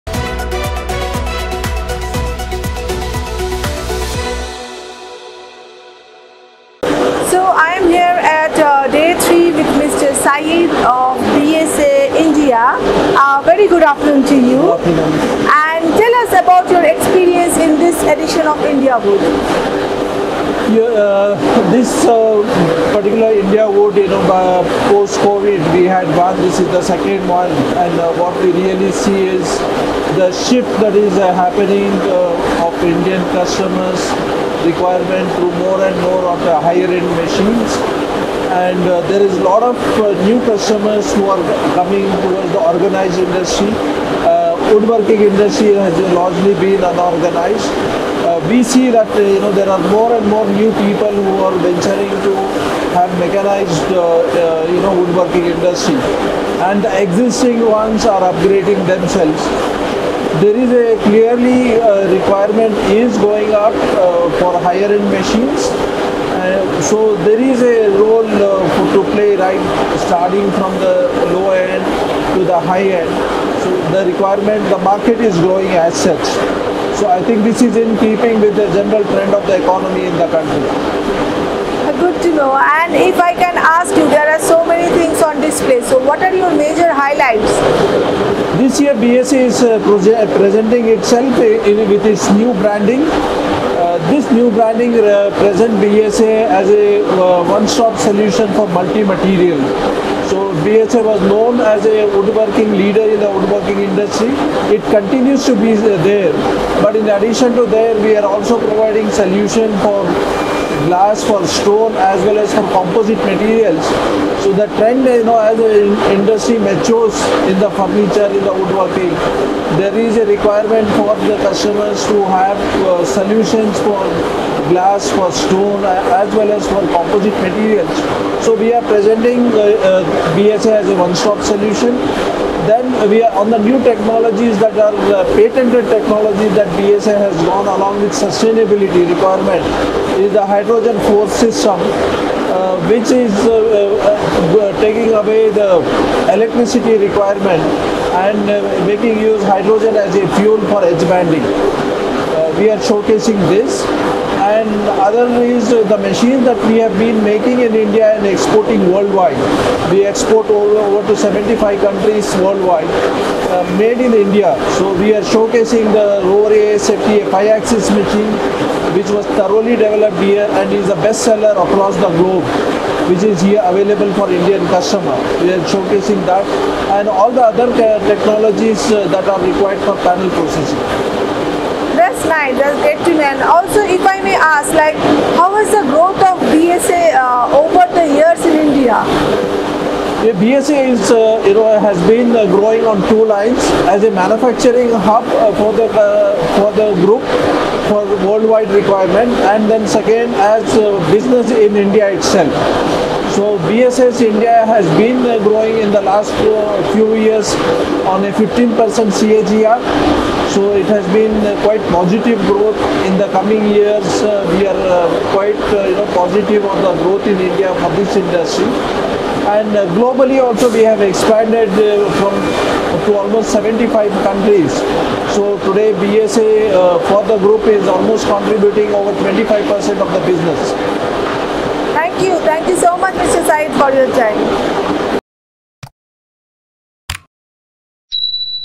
Biesse showcases itself as a one stop solution for multi-material at IndiaWood
Sayeed Ahmed, CEO of Biesse India shares his experiences to be at IndiaWood, showcasing latest technologies and witnessing the shift in the mindset of Indian customers in accepting more and more high end technologies and more, in an exclusive interview with Wood & Panel.